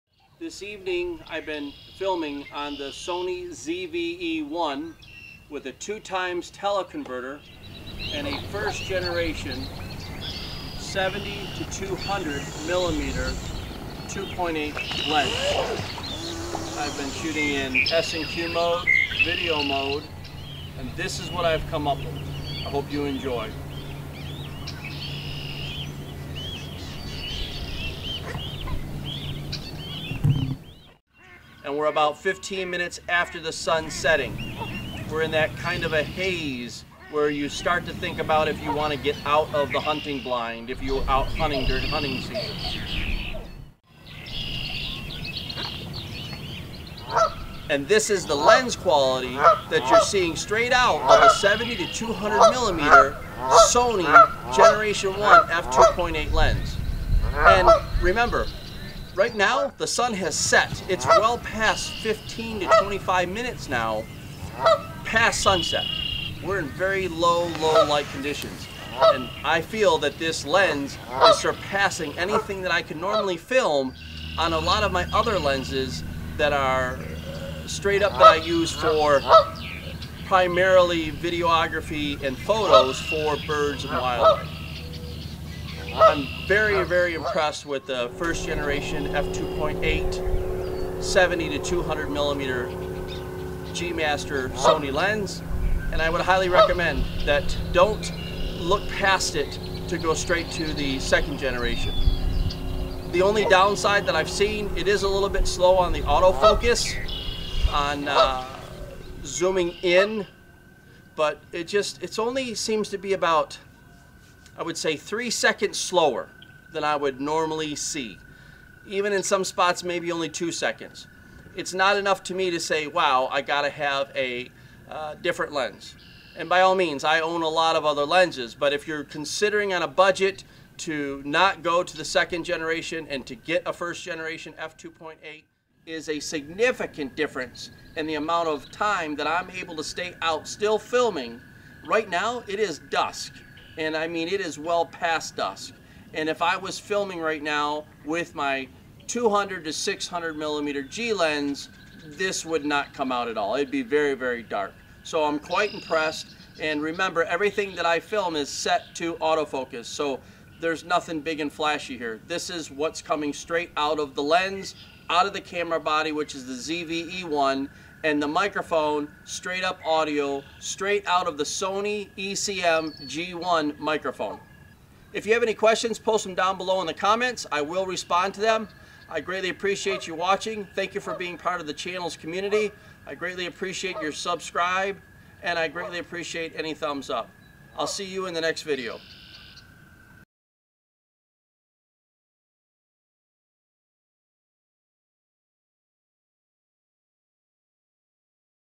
Sony ZV-E1 | Bird Autofocus In Low Light | Sony 70-200mm F/2.8 Gm Lens | Cinev
In this video I had a chance to take out the new Sony ZV-E1 and connect it to the Sony 70-200mm F2.8 Lens and pair it up with the Sony ECM-G1 Microphone and capture some great set to bird recognition autofocus while filming Swans on the CineVlog setting of the Sony ZV-E1 at sunset.